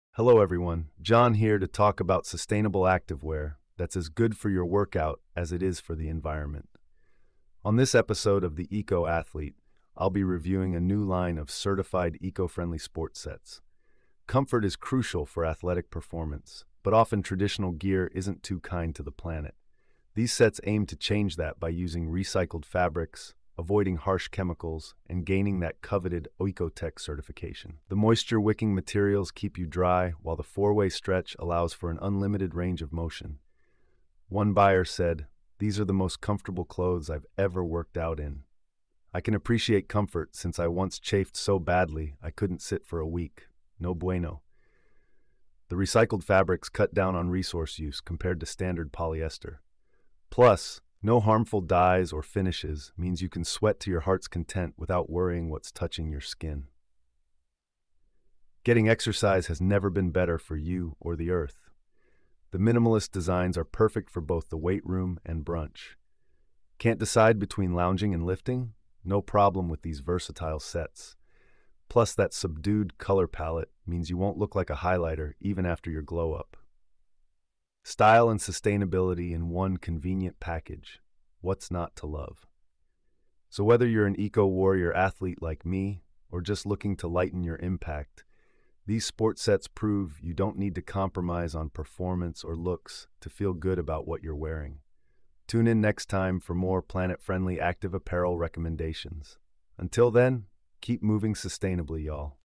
Comfort Meets Sustainability: Unleash Your Athletic Potential with Eco Friendly Sport Sets
Sustainable Activewear That Performs

Join host John as he reviews the latest eco-friendly sportswear on The Eco Athlete podcast. Each episode explores gear that empowers your workouts while protecting the planet.
 This Week's Show

In the debut episode, John introduces a new line of certified sustainable sport sets made from recycled fabrics. Learn how their design boosts comfort and mobility for maximum athletic ability.

Hear details on the performance fabrics, minimized impact manufacturing and versatile styles. Get tips for integrating the sets into your workout routine while reducing textile waste. Whether you're an elite athlete or just getting active, this show is for you.